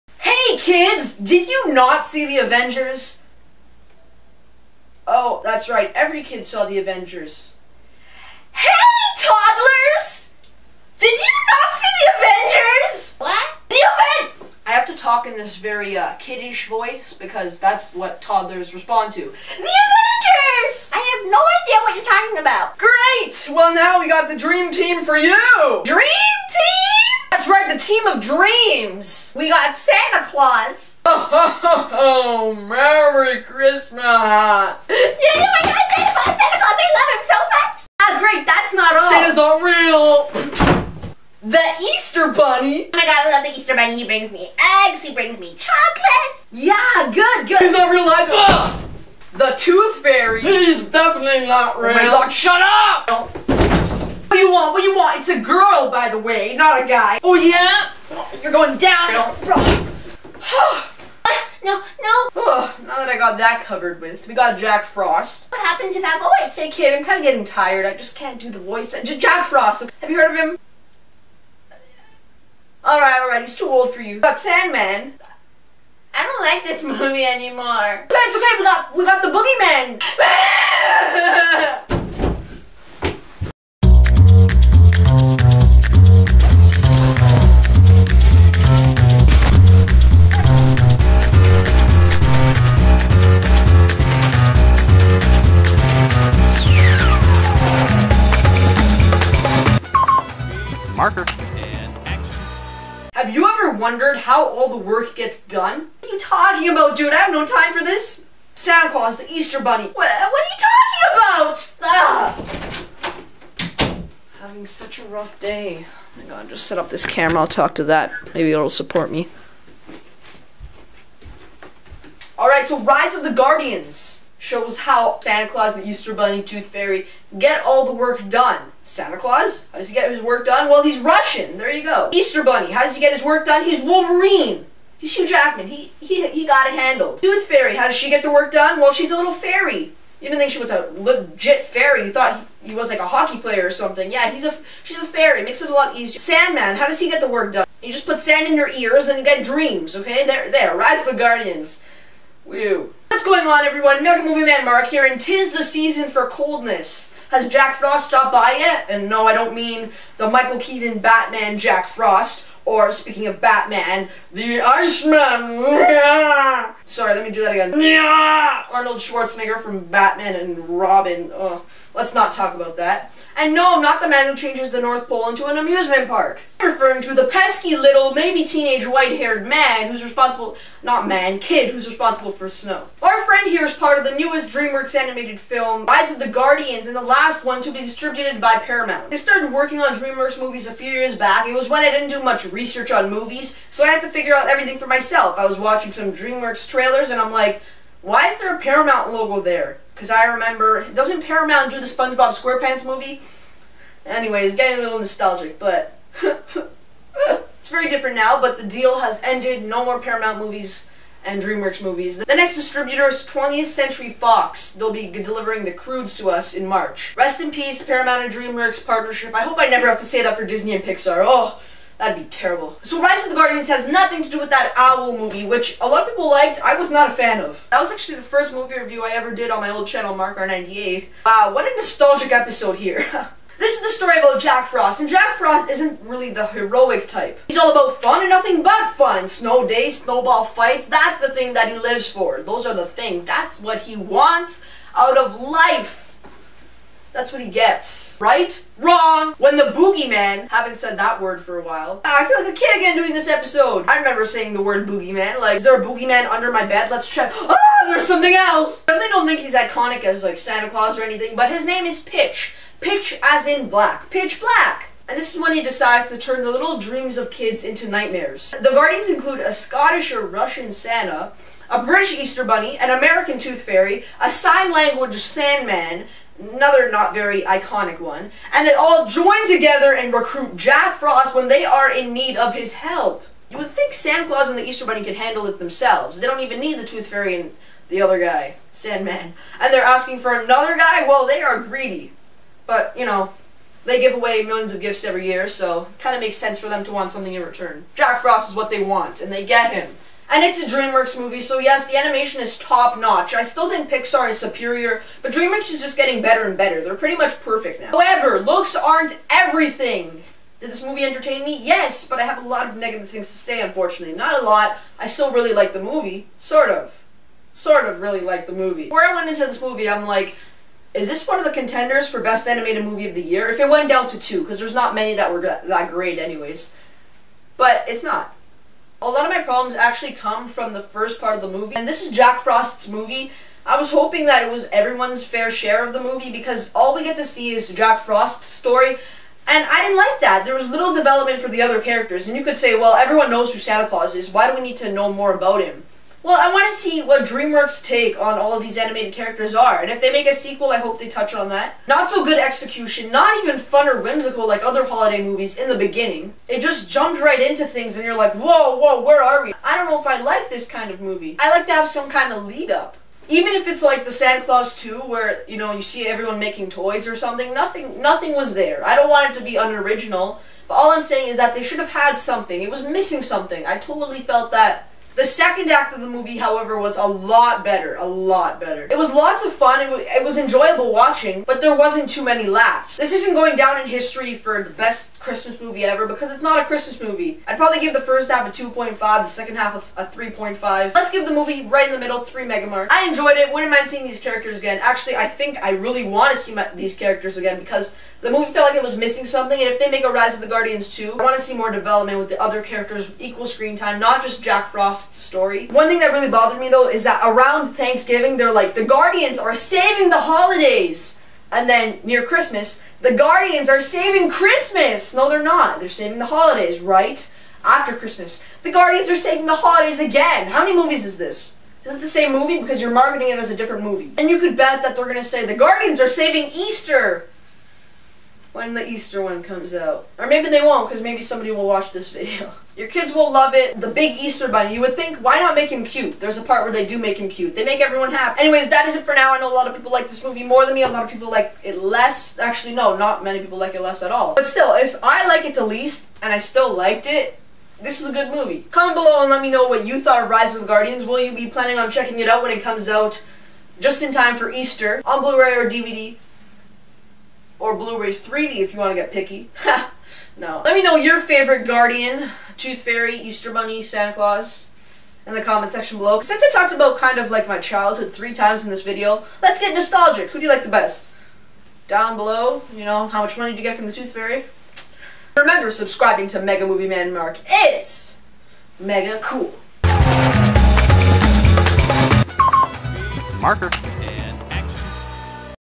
RISE OF THE GUARDIANS PARODY + MEGA Movie Review
MegaMovieManMark rise to deliver his Mega Review and Spoof for the last Dreamworks film distributed by Paramount, Rise of the Guardians. Featuring Santa Claus, Easter Bunny, Tooth Fairy, Sandman, Jack Frost and The Boogeyman, "Rise of the Guardians" is available now, but question is- Should you buy it??